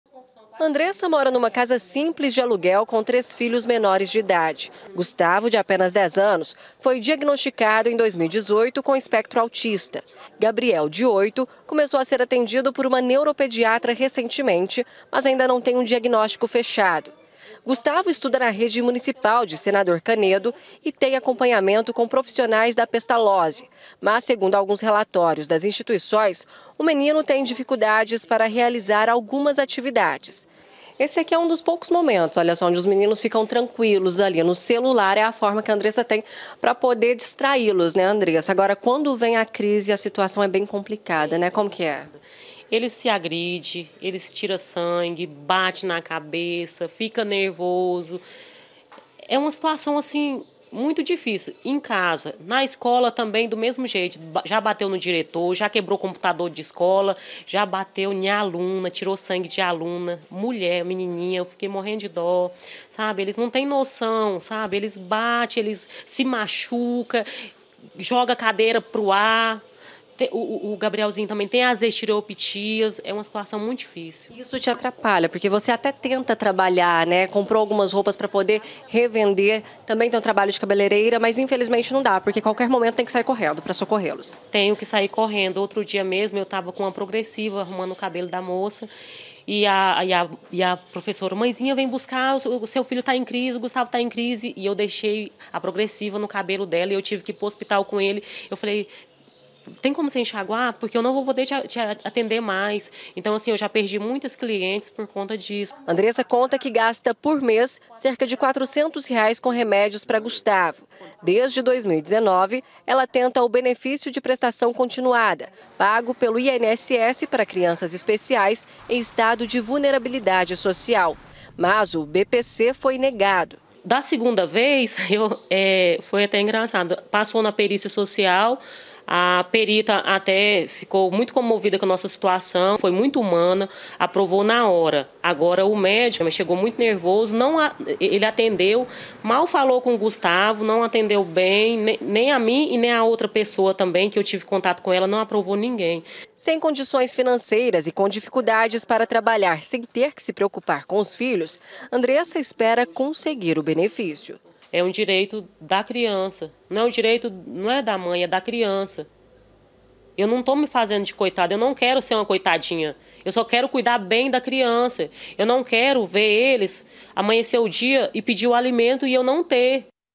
EM BUSCA DE UM BENEFÍCIO: MÃE AGUARDA RESPOSTA DO INSS DESDE 2019
A MÃE DE UM MENINO AUTISTA ESTÁ LUTANDO, DESDE 2019, PARA CONSEGUIR O BENEFÍCIO DO INSS PARA CUIDAR DA FAMÍLIA. POR CAUSA DAS CONSTANTES CRISES DO FILHO, ELA NÃO CONSEGUE TRABALHAR E TEM PASSADO POR DIFICULDADES.